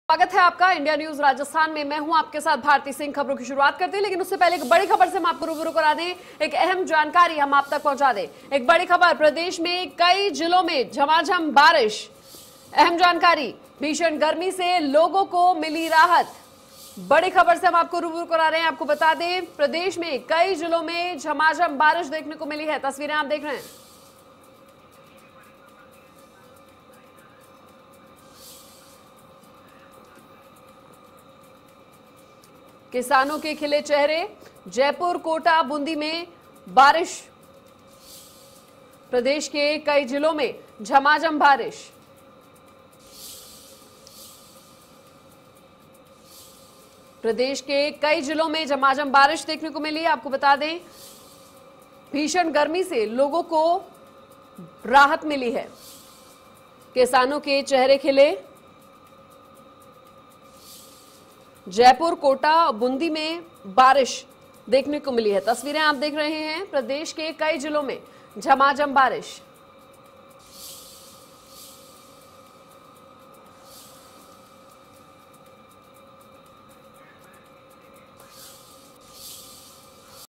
Rajasthan News : प्रदेश के कई जिलों में झमाझम बारिश- किसानों के चेहरे पर दिखी रौनक
Rajasthan News : प्रदेश के कई जिलों में झमाझम बारिश- किसानों के चेहरे पर दिखी रौनक  India News Rajasthan